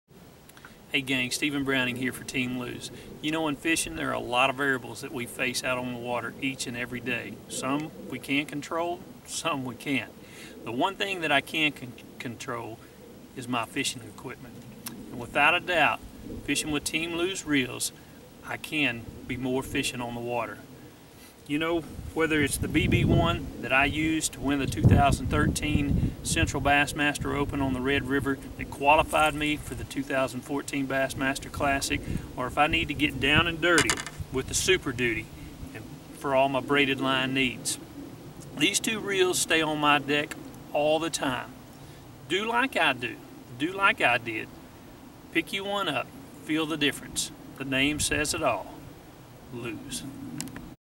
Stephen Browning talks about why he uses Lew's
National Lew's Pro Stephen Browning talks about why he uses Lew's reel for his success on the Bassmaster Elite Series.